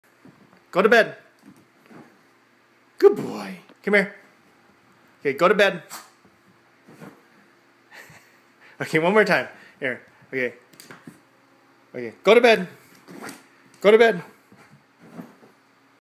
Bailey goes to bed!
Bailey does his go to bed trick for Kyla!